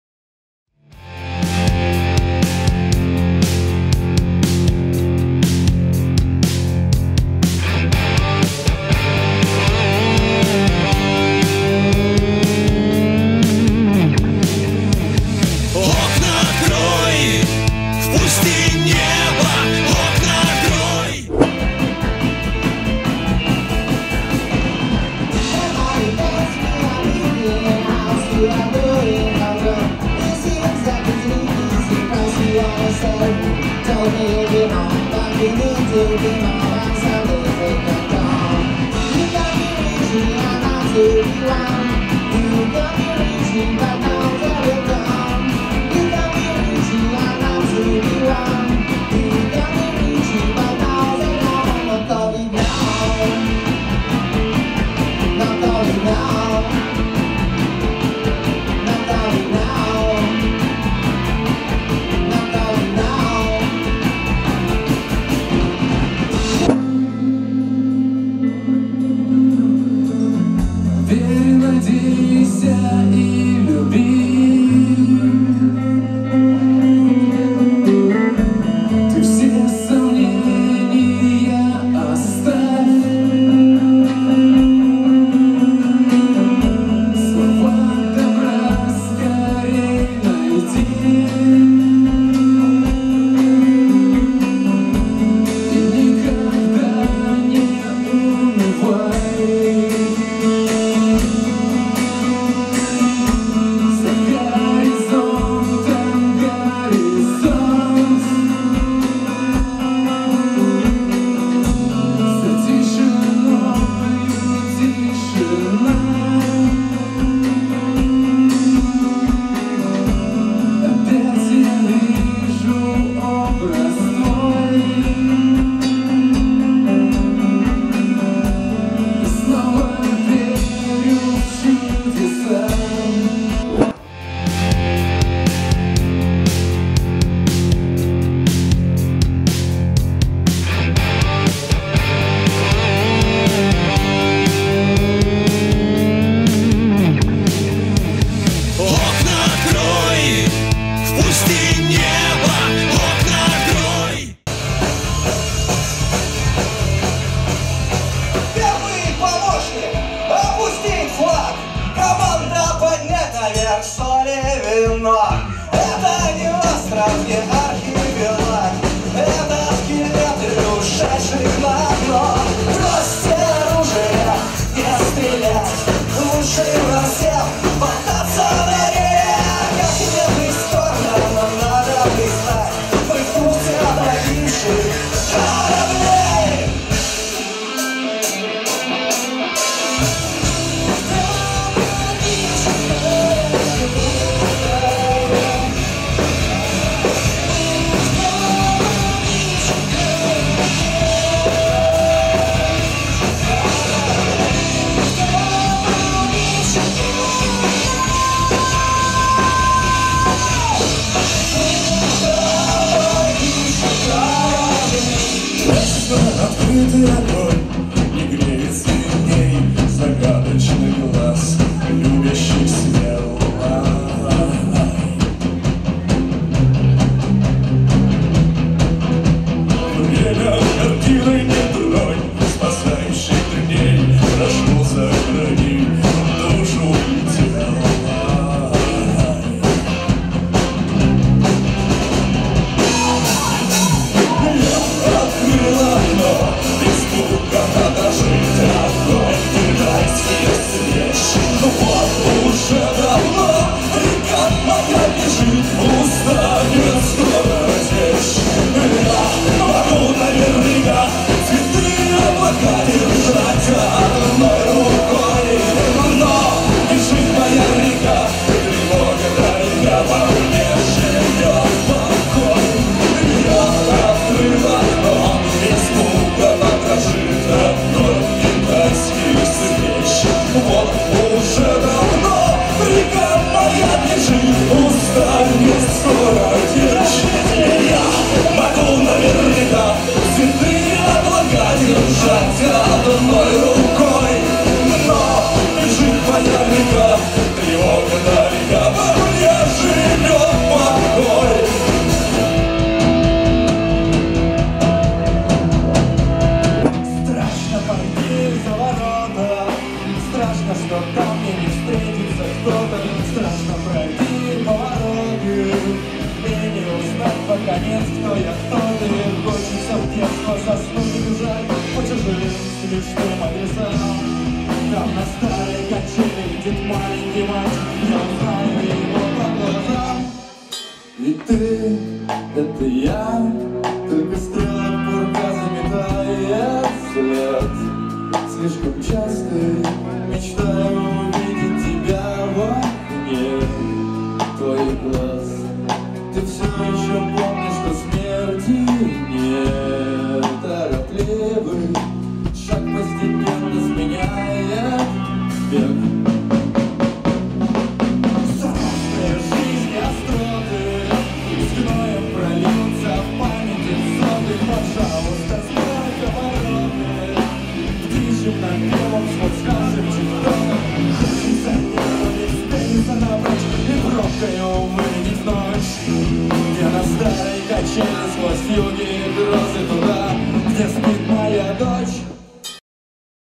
Ролик с пятого отборочного тура фестиваля «ОКНА ОТКРОЙ» - 2024.
5 марта состоялся пятый отборочный тур фестиваля ОКНА ОТКРОЙ в клубе Zoccolo 2.0 (СПб).
В пятом отборочном играли :
1. Die Spiele =DDD
2. Седьмая ценность
3. Последний пионер
4. Сердце Тру
5. Привет, Бендер!